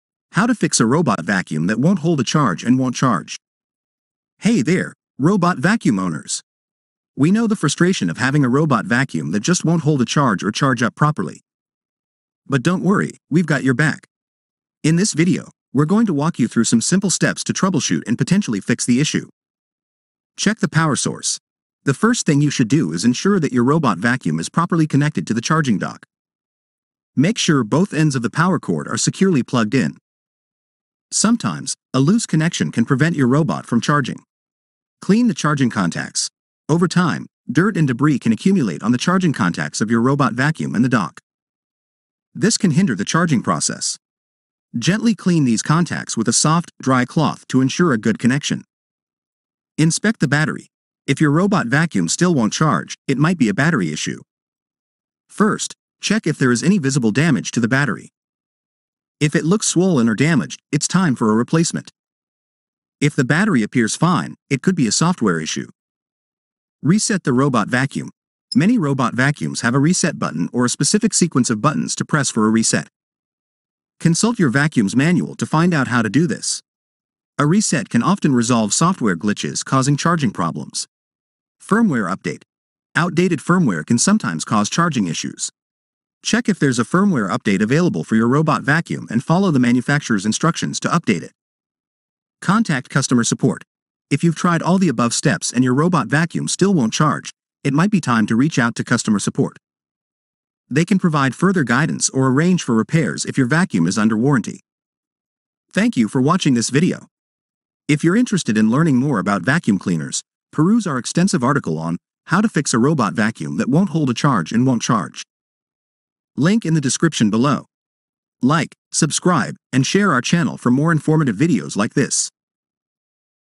Robot Vacuum Not Charging? Easy Fixes That Work
Is your robot vacuum not charging or refusing to hold a charge? This video shares simple solutions to get it back on track fast. Learn what causes charging issues and how to fix them with ease. Say goodbye to frustrating power problems!